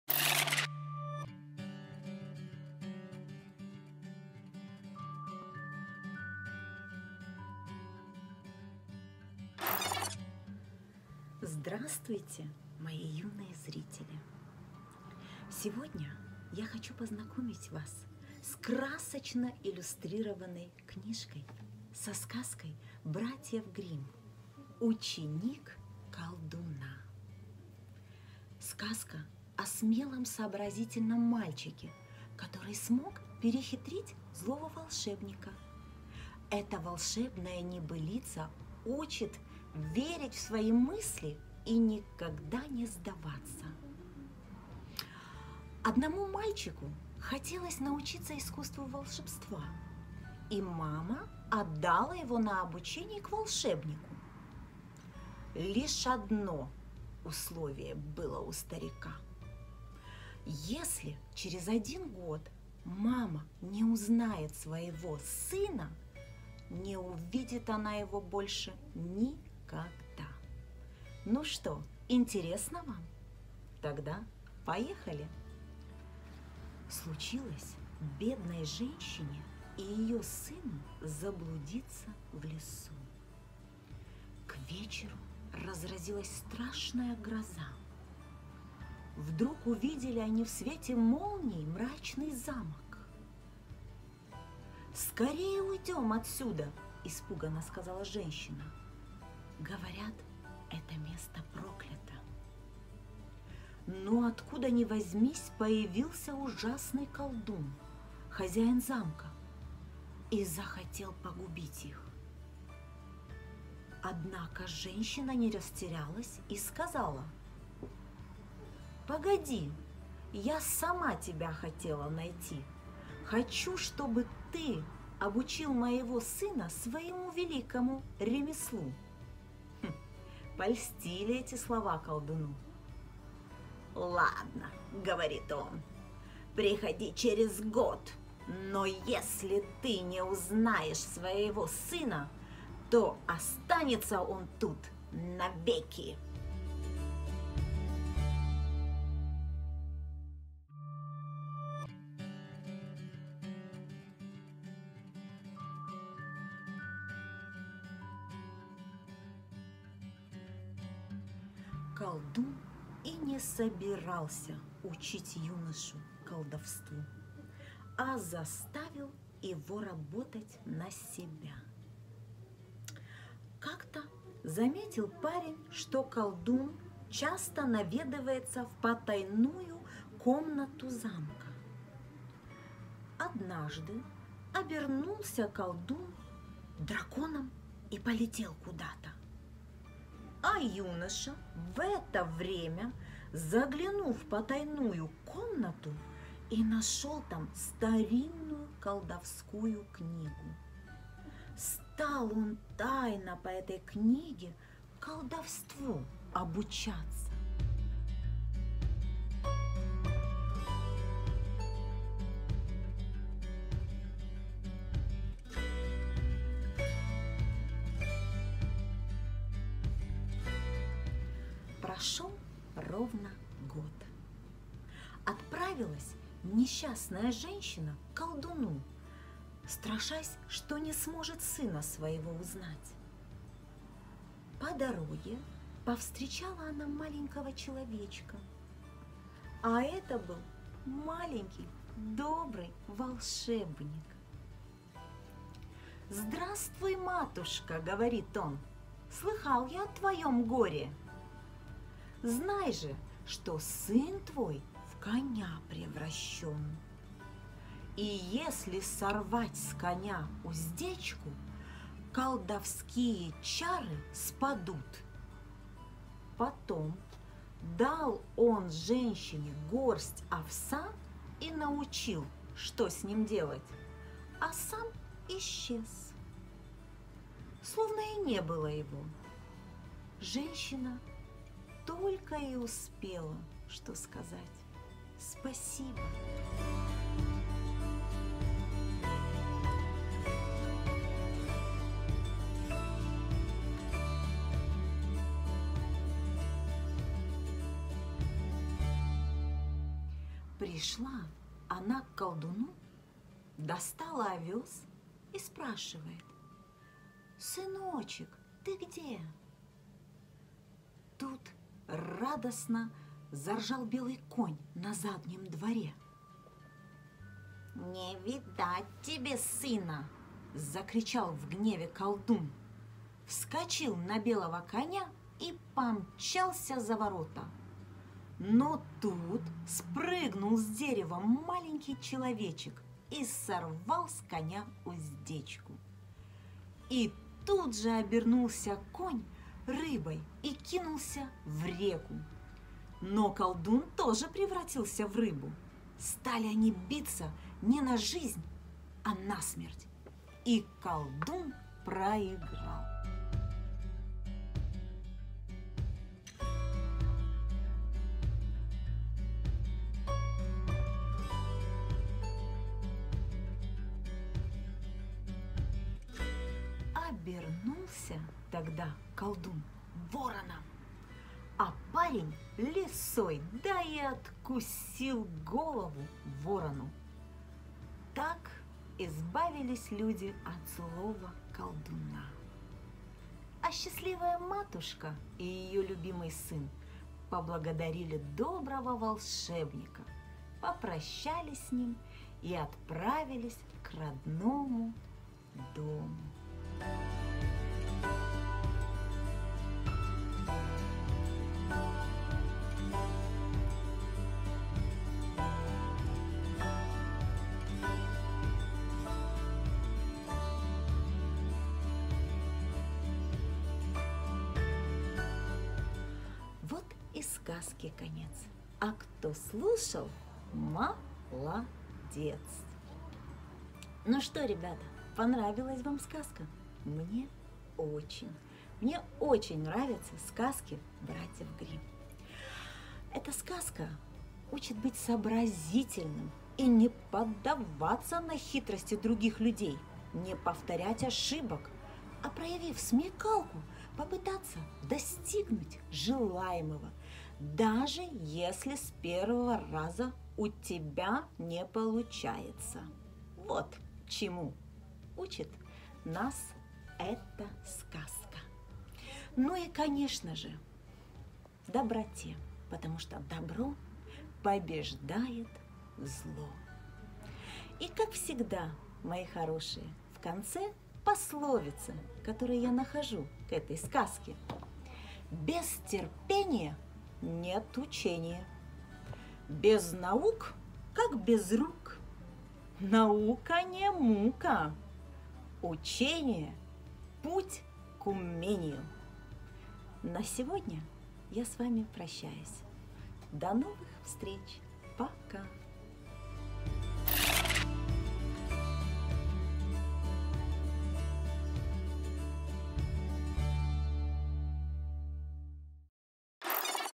Ученик Колдуна
Сказка „Ученик колдуна”, о  смелом и сообразительном мальчике, который смог перехитрить злого волшебника , эта волшебная небылица учит  верить в свои силы и никогда не сдаваться.  Для вас читает Лилиана Манойлова